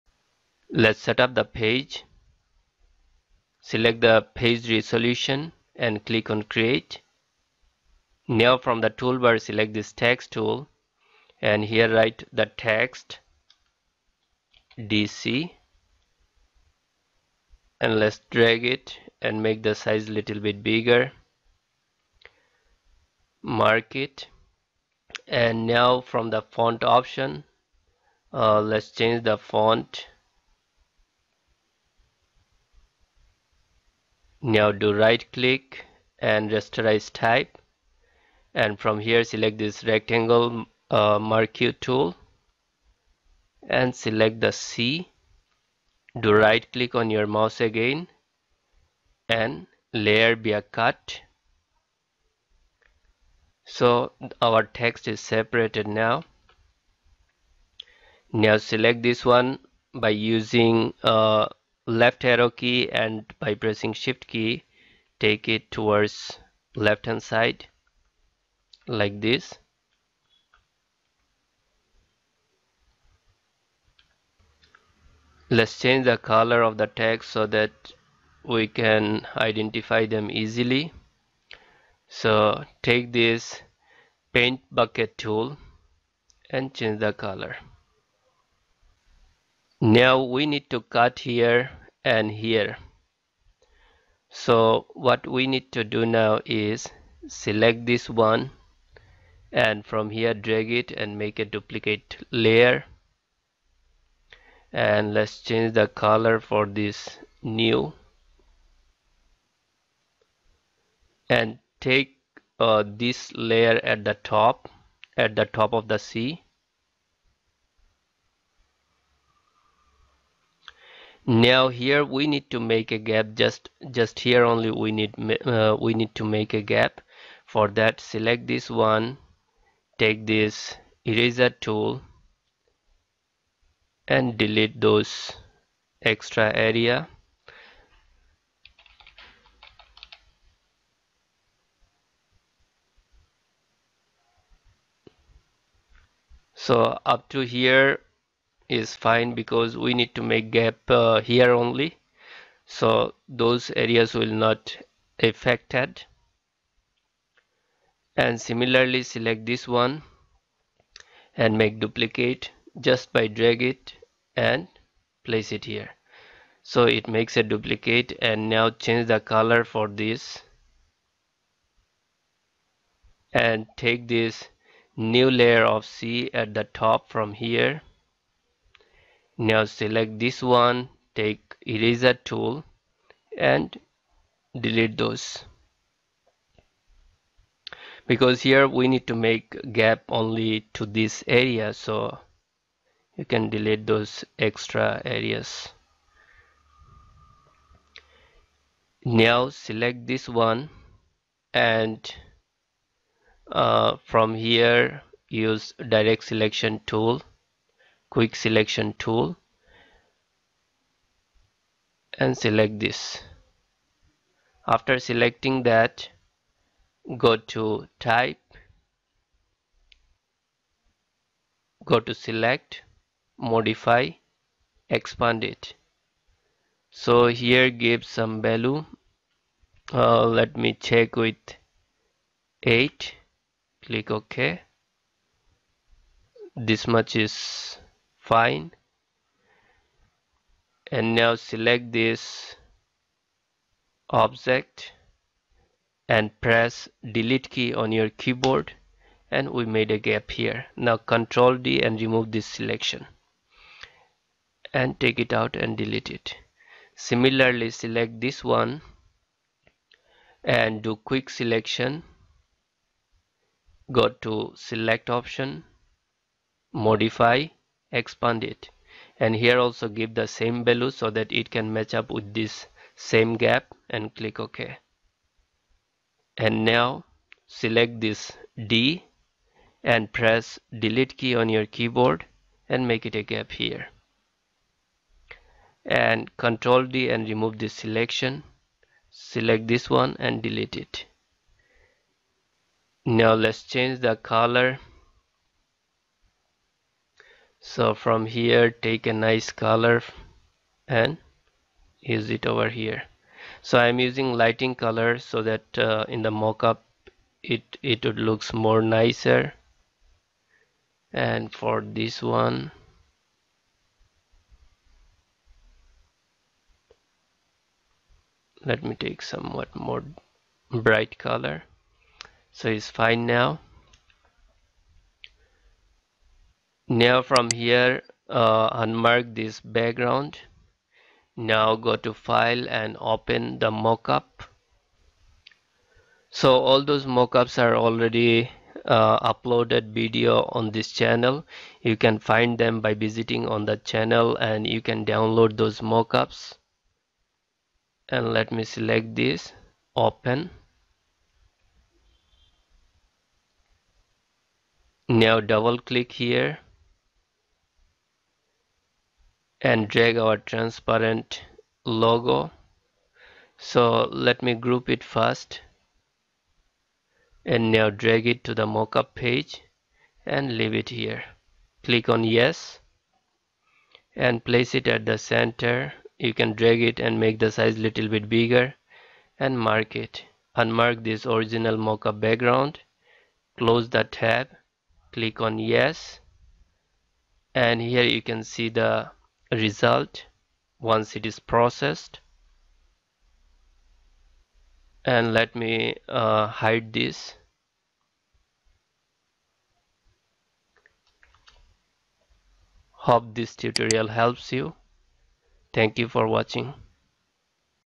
Photoshop Logo Design Tutorial || How to make DC text logo in photoshop cc and apply mockup to it
👇Download all your graphic design needs from one website, Grab it now !